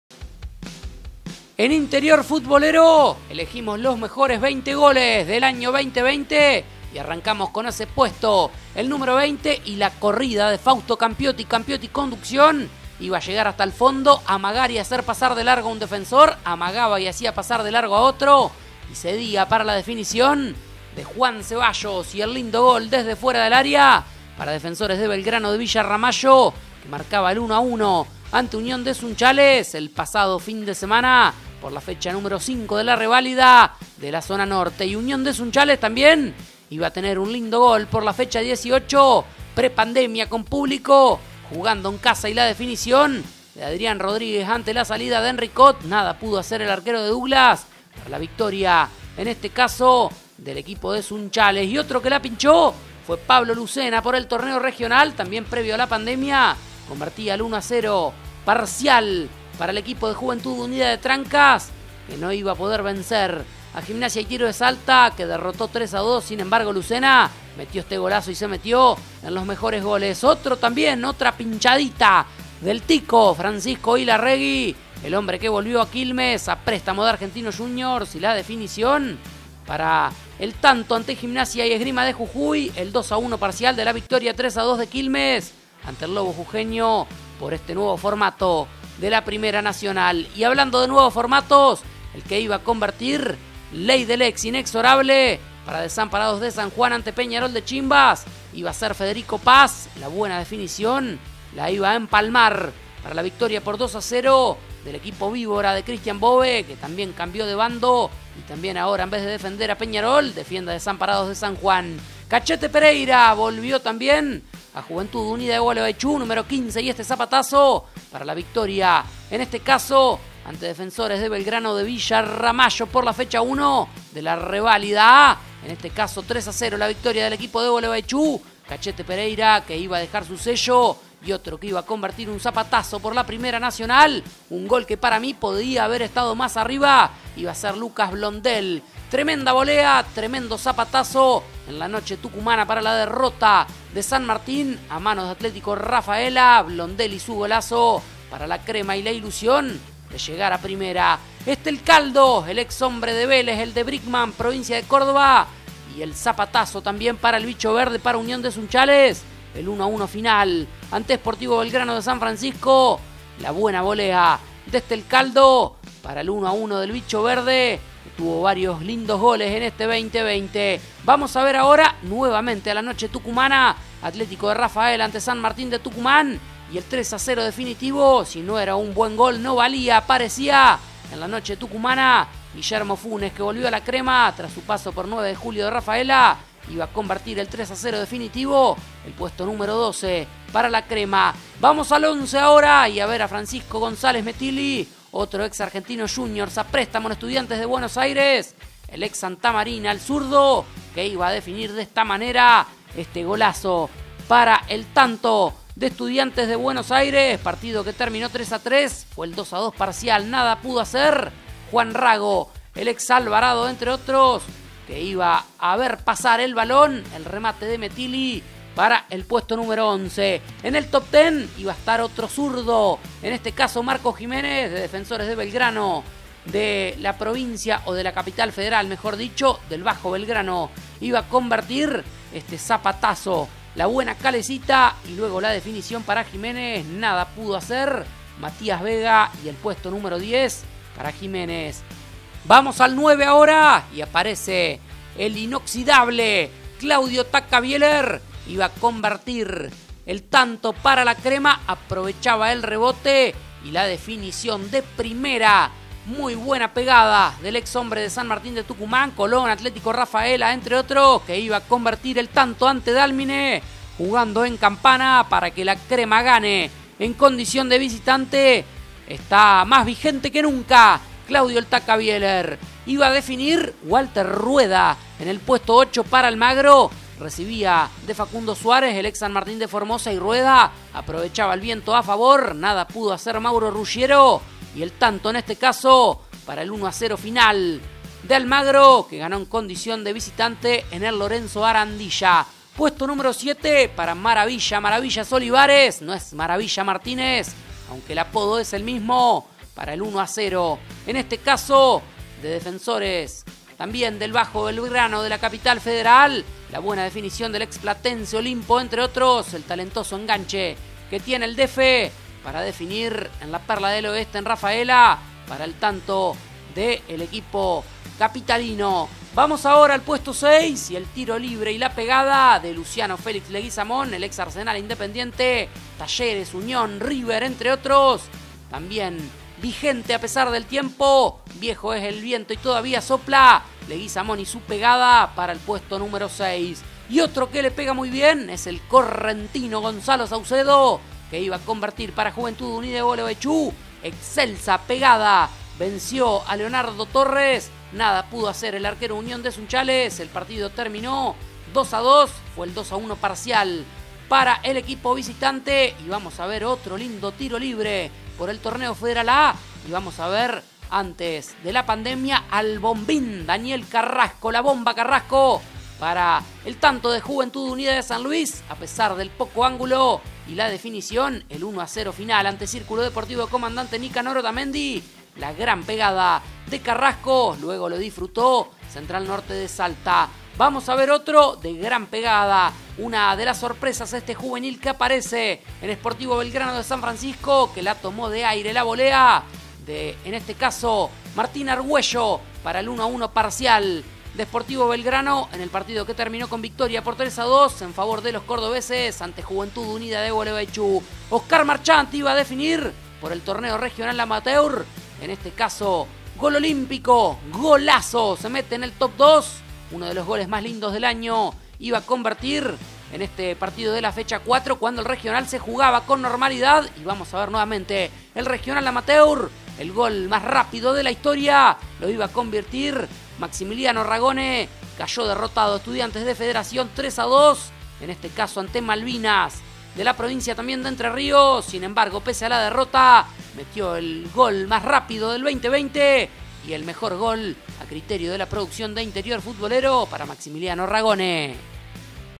Mejores goles del 2020 del fútbol del interior: Primera Nacional, Federal A y Regional Amateur
Los mejores goles del año de todo el fútbol del interior:

20) JUAN CEBALLOS (DEFENSORES VR)
19) MARTÍN RODRÍGUEZ - UNIÓN (SUNCHALES)
18) PABLO LUCENA (JUVENTUD UNIDA DE TRANCAS)
17) FRANCISCO ILARREGUI (QUILMES)
16) FEDERICO PAZ (DESAMPARADOS)
15) JUAN FRANCISCO FERREYRA (JUVENTUD UNIDA GUALEGUAYCHÚ)
14) LUCAS BLONDEL (ATLÉTICO RAFAELA)
13) SANTIAGO STECALDO (UNIÓN SUNCHALES)
12) GUILLERMO FUNES (ATLÉTICO RAFAELA)
11) FRANCISCO GONZÁLEZ METILLI (ESTUDIANTES BA)
10) MARCOS GIMÉNEZ (DEFENSORES DE BELGRANO)
9) CLAUDIO BIELER (ATLÉTICO RAFAELA)
8) WALTER RUEDA (ALMAGRO)
7) JUAN MANUEL OLIVARES (DEFENSORES DE BELGRANO)
6) LUCIANO LEGUIZAMÓN (GIMNASIA CDU)
5) GONZALO SAUCEDO (JUVENTUD UNIDA G)
4) DANIEL CARRASCO (JUVENTUD UNIDA SL)
3) MARTÍN ARGÜELLO (SPORTIVO BELGRANO)
2) OSCAR MARCHANT (JORGE NEWBERY CR)
1) MAXIMILIANO RAGONE (ESTUDIANTES DE FEDERACIÓN)